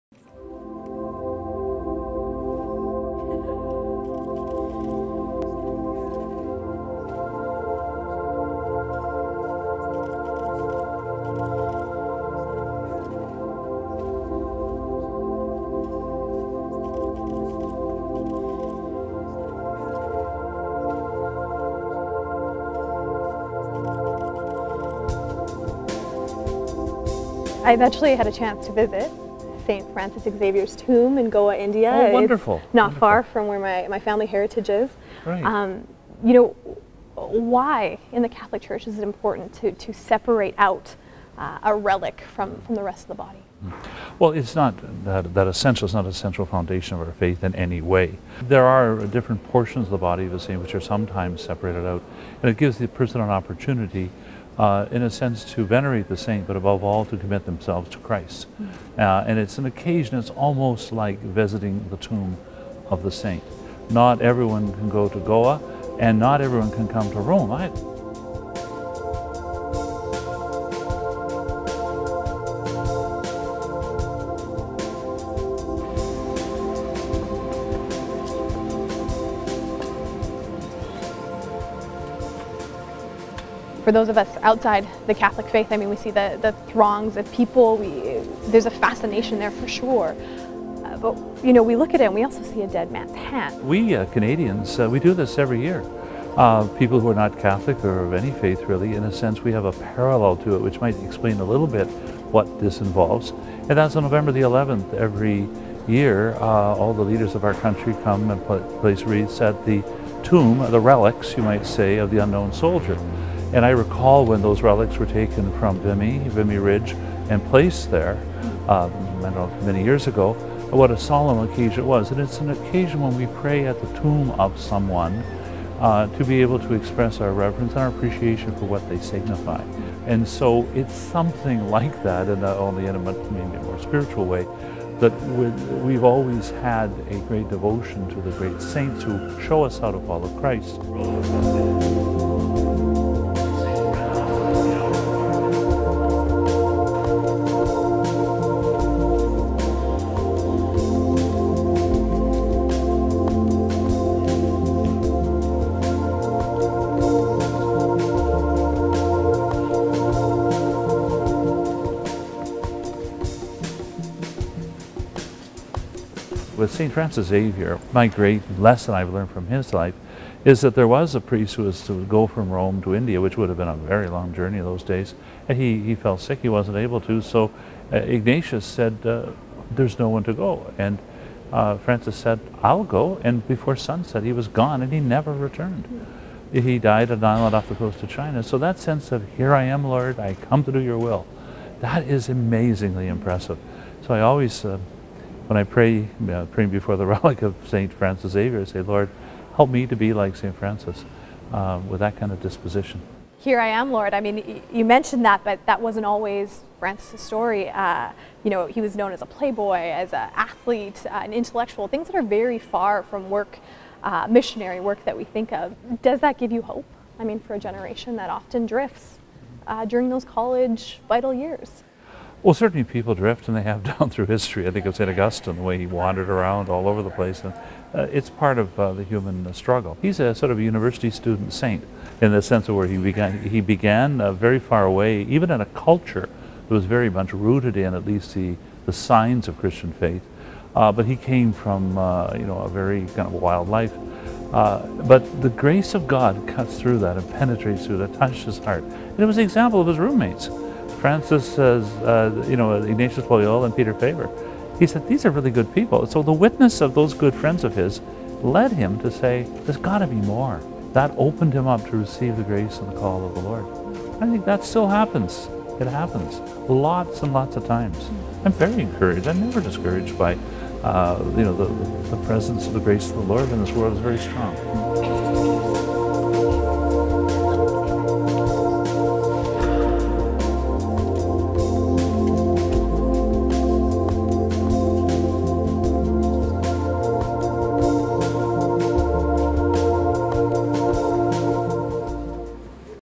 The “Miracle Hand” through the lens of Cardinal Collins
As Saint Francis Xavier's 465-year-old hand and forearm tour Canada, Molly Thomas spoke with His Eminence, Cardinal Collins about its meaning and purpose in 2018.